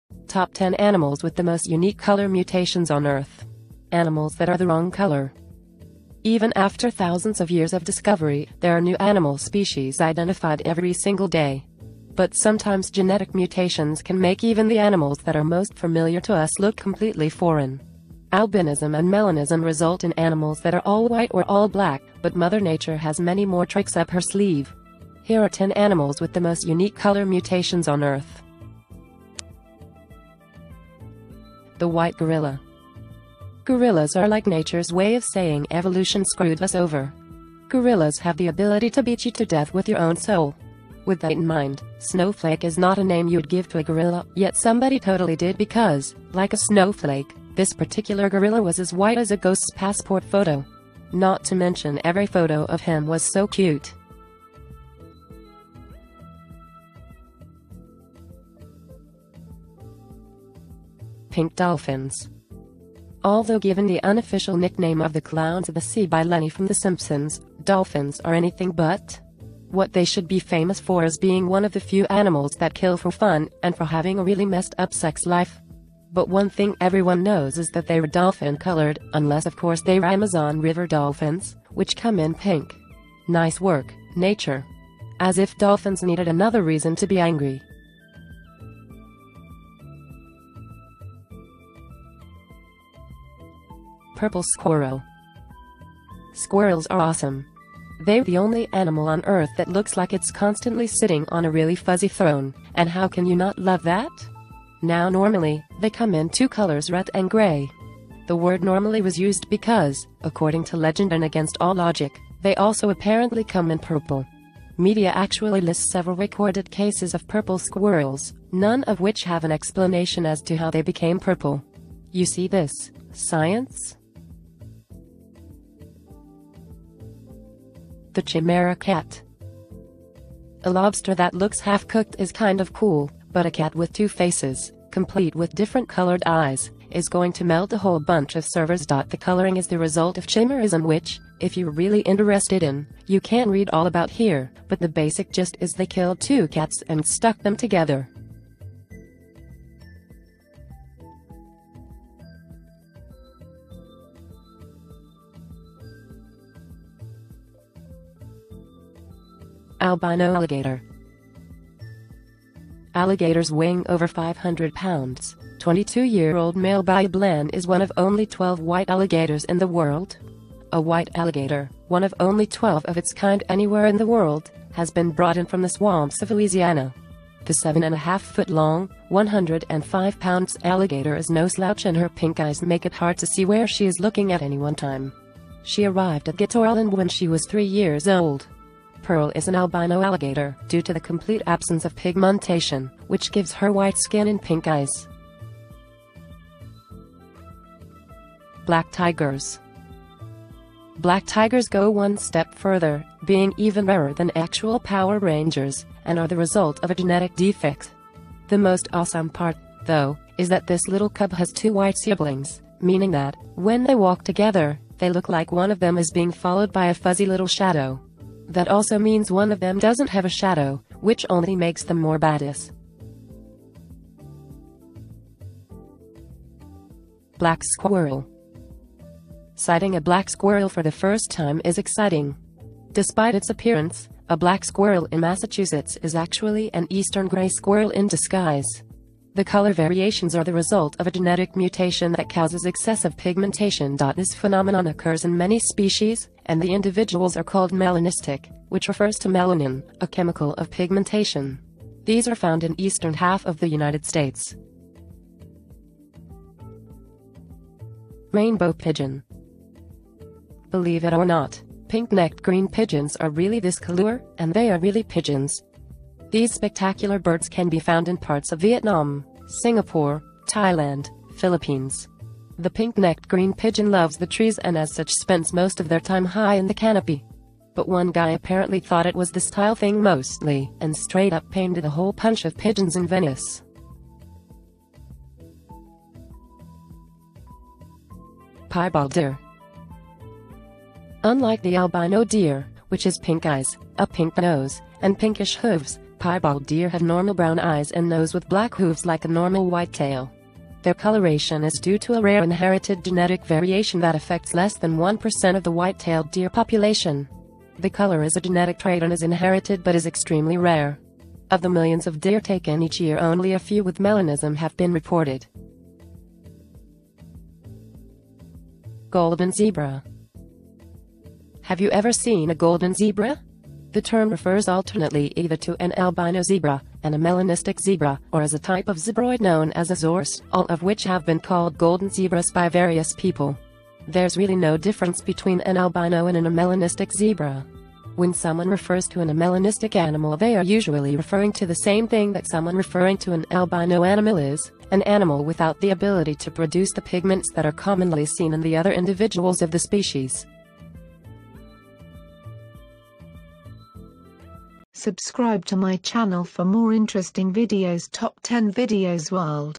Top 10 Animals With The Most Unique Color Mutations on Earth
Top 10 Animals With The Most Unique Color Mutations on Earth   Animals That Are The Wrong Color
Even after thousands of years of discovery, there are new animal species identified every single day. But sometimes genetic mutations can make even the animals that are most familiar to us look completely foreign.  Albinism and Melanism result in animals that are all-white or all-black, but mother nature has many more tricks up her sleeve. Here are 10 Animals With The Most Unique Color Mutations On Earth.